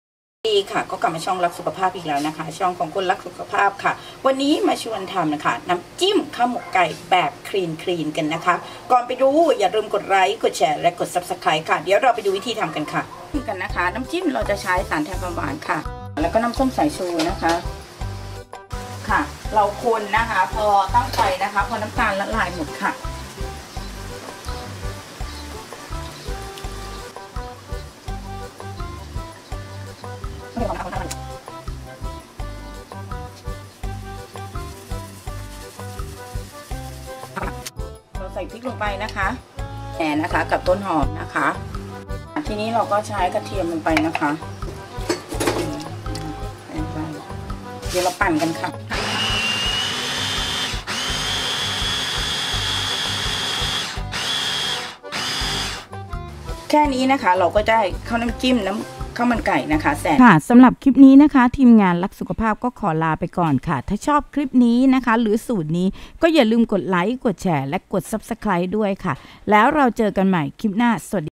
แจกสูตรน้ำจิ้มข้าวหมกไก่คลีน ทำง่ายอรอ่ยมาก
ส่วนผสมน้ำจิ้ม
1.สารแทนความหวาน 1/2 ถ้วยตวง
2.น้ำส้มสายชู 1/2 ถ้วยตวง
3.พริกจินดา 4 เมล็ด
4.ใบสาระเหน่
5.ต้นหอมซอย
6.กระเทียมจีน 3 กลีบ
Sauce ingredients
1. 1/2 cup sweetener
2.1 / 2 cup vinegar
3. Chili Chinda, 4 seeds
4. Mint leaves
5. scallion
6.3 cloves of Chinese Garlic